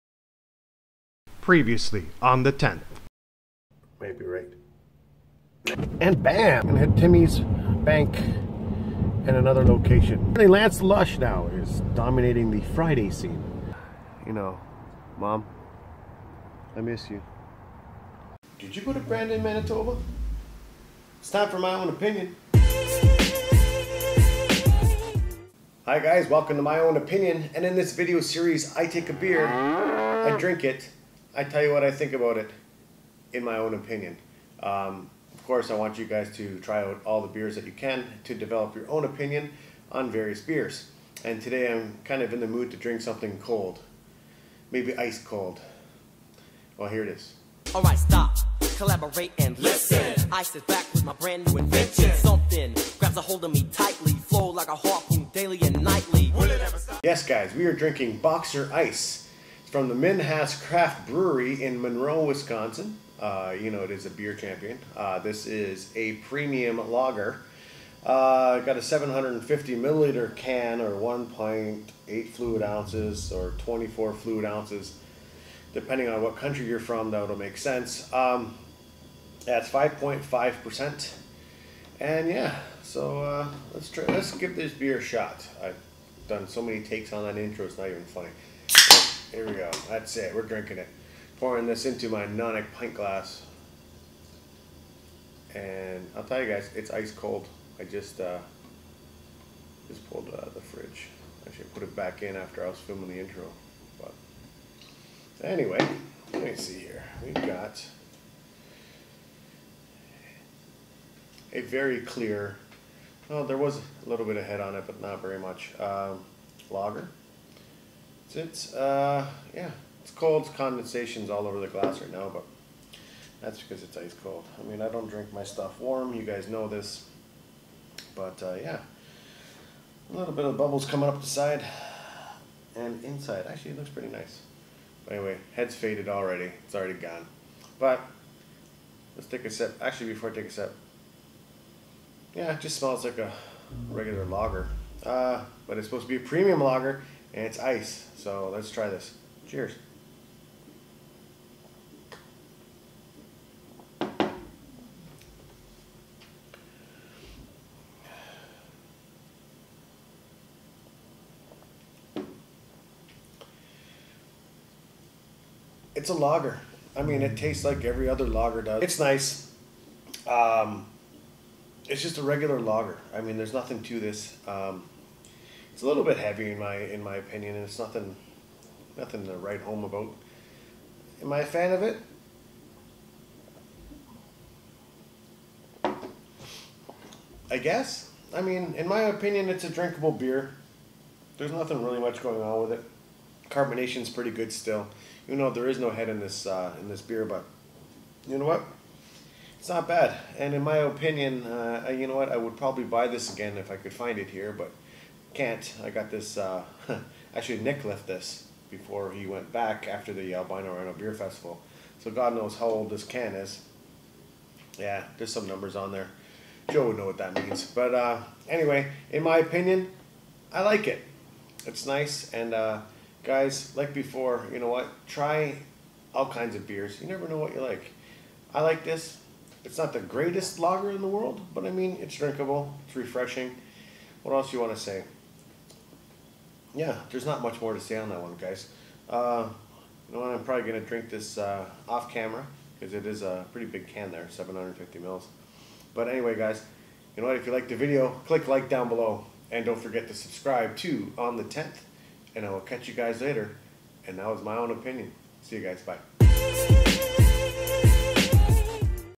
On The 10th - My Own Opinion - Boxer Ice 5.5%
Today on My Own Opinion, I drink Boxer Ice from the Minhas Craft Brewery in Monroe, WI.

Do I like it? Doesn't matter. It's My Own Opinion